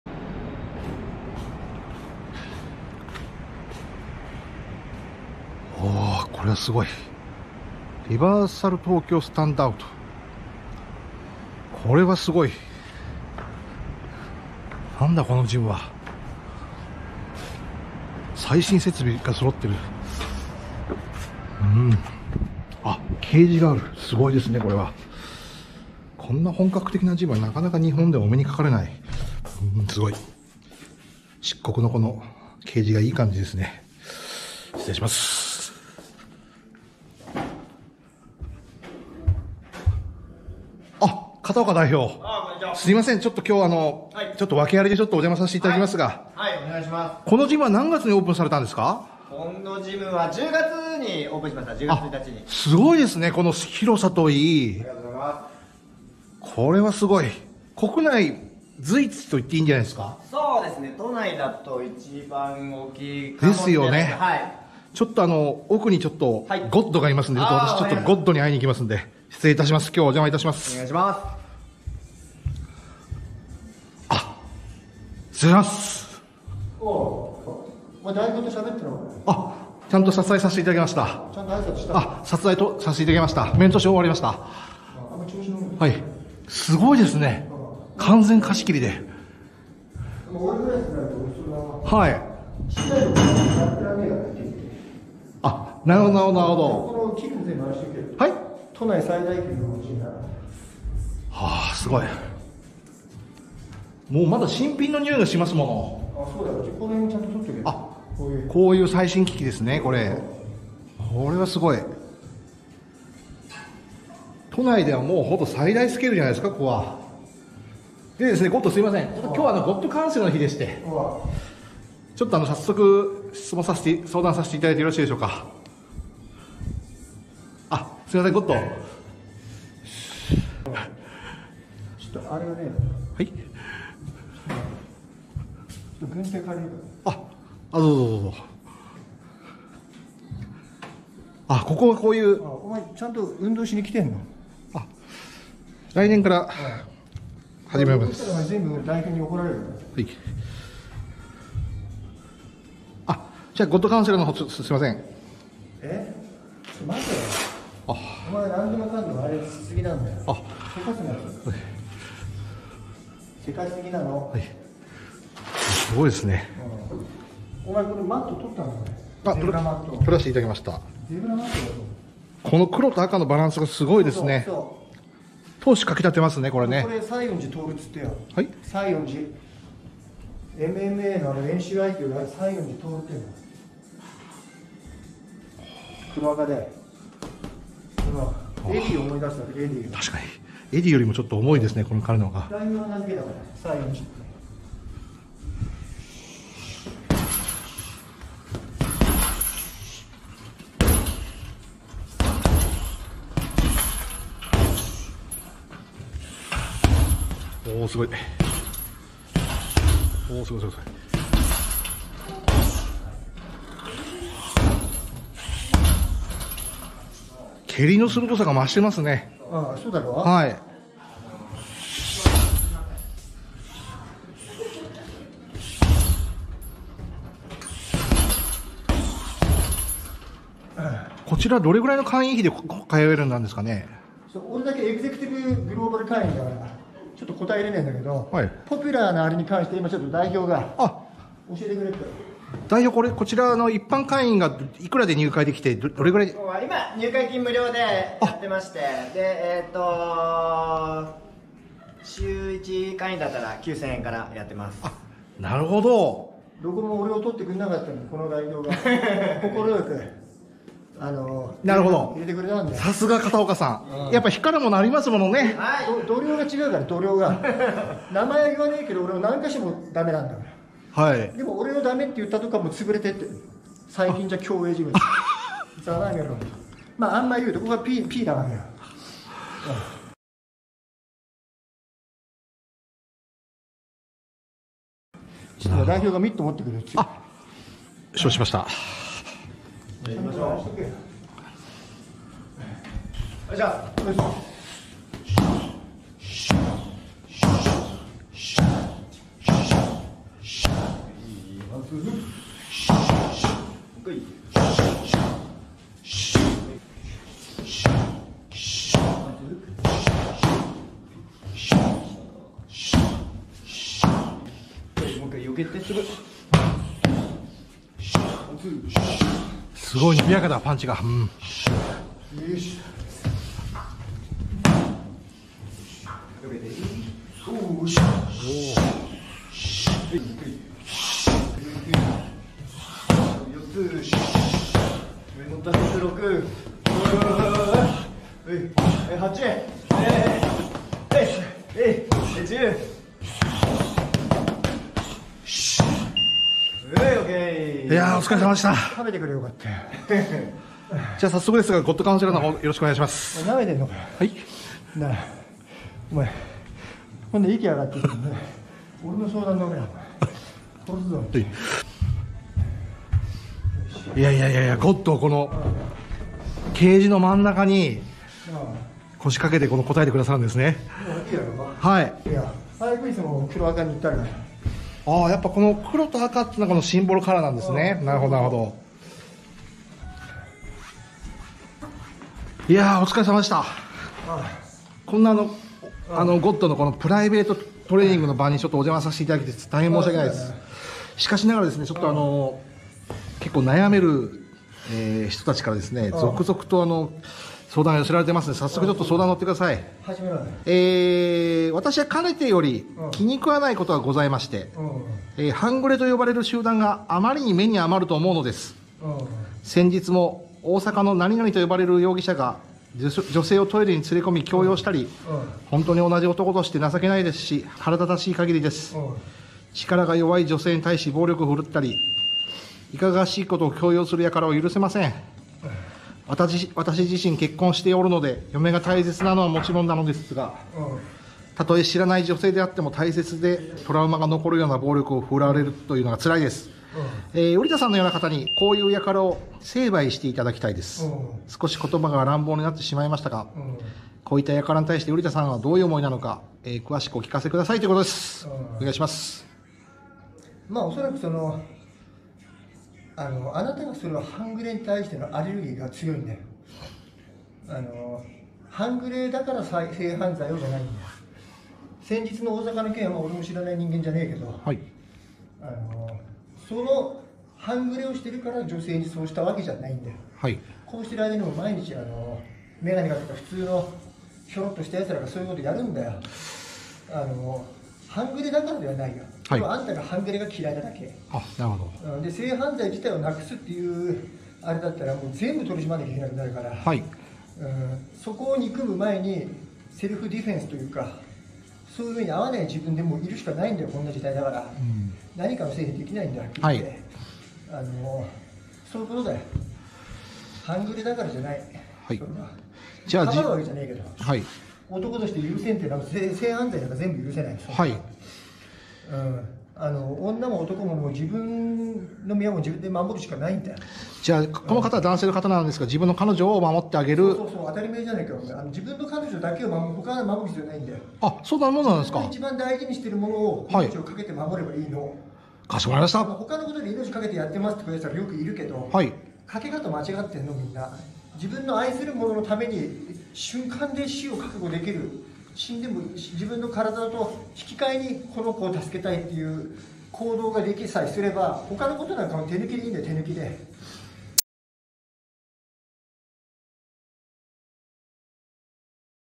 【半グレの性犯罪】許せない気持ちは分かるよ
相談内容もさることながらサンドバッグを叩いてる瓜田氏
格好良いです